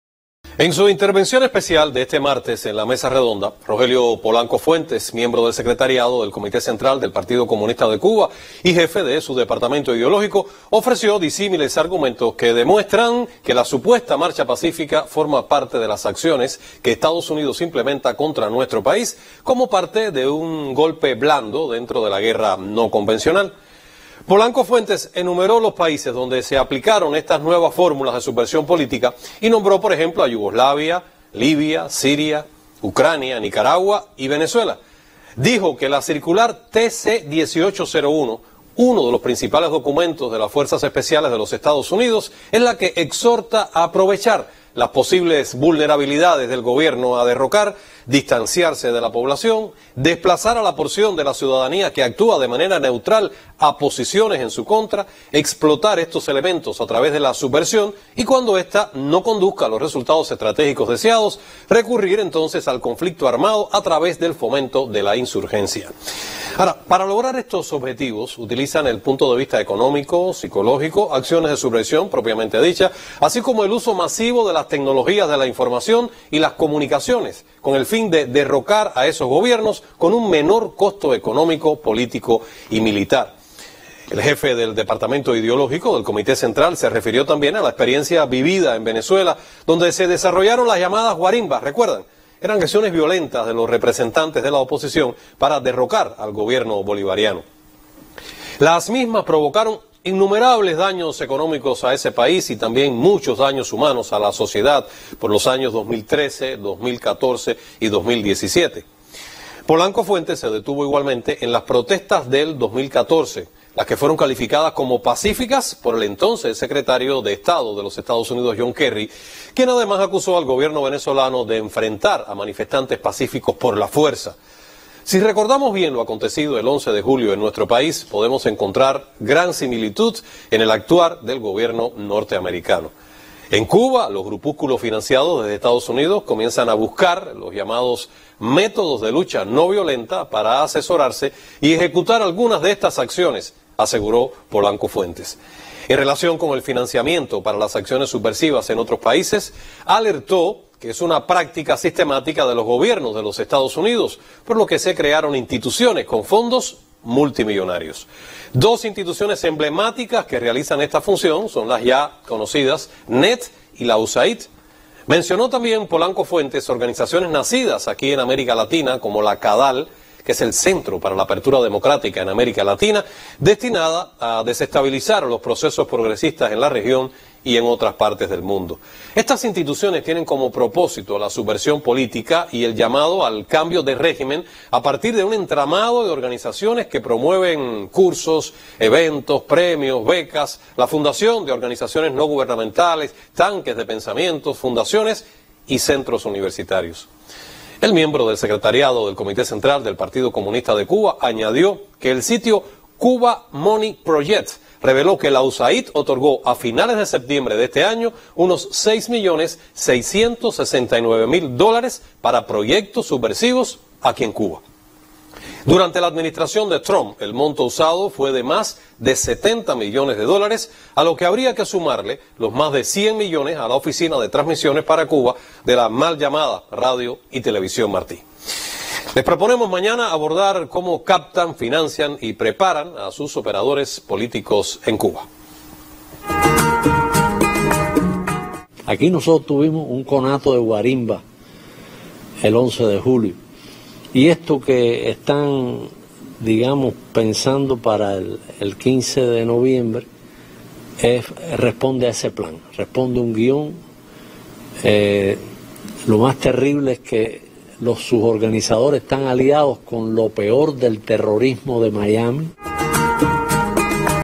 Expone Cuba argumentos sobre la supuesta marcha pacífica como acción subversiva de EE.UU.
En su intervención especial de este martes en la Mesa Redonda, Rogelio Polanco Fuentes, Miembro del Secretariado del Comité Central del Partido Comunista de Cuba y Jefe de su Departamento Ideológico ofreció disimiles argumentos que demuestran que la supuesta MarchaPacífica forma parte de las acciones que EstadosUnidos implementa contra nuestro país como parte de un golpe blando dentro de la Guerra No convencional.